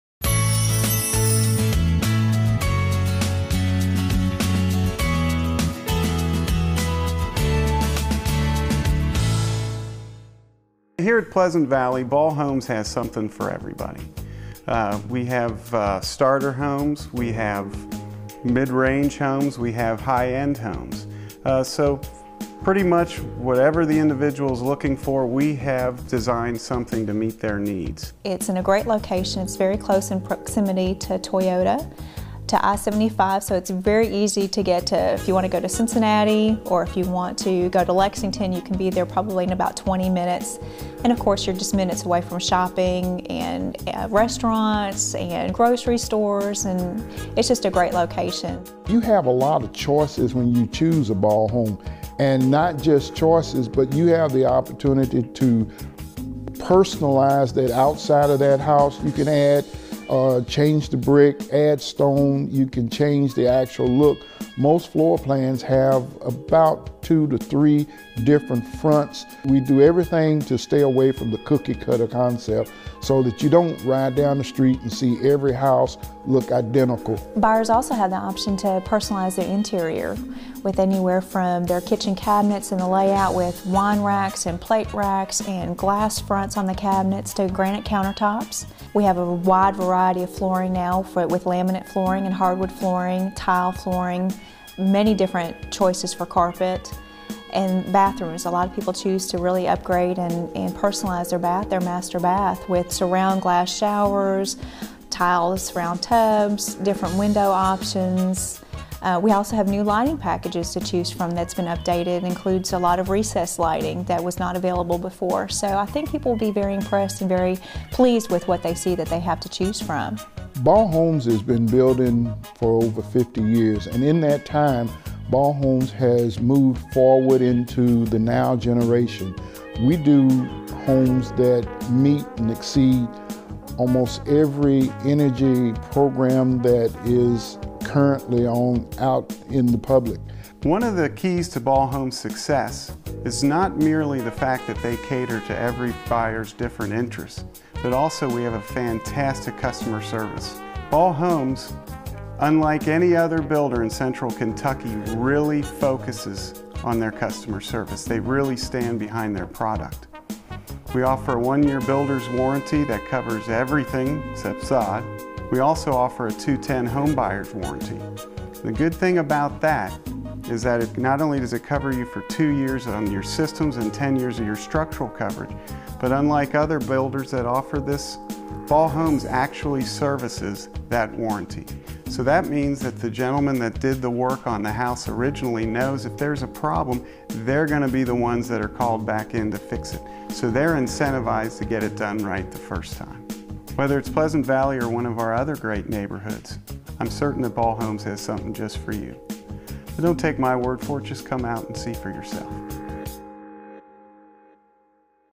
Ball Homes - Pleasant Valley Neighborhood Tour
The Landing at Pleasant Valley is a Ball Homes subdivision located near Toyota Manufacturing plant of Georgetown, and is convenient I-75.  The neighborhood is close to the Cherry Blossom Golf and Country Club, and shopping, dining, and conveniences on Cherry Blossom Way.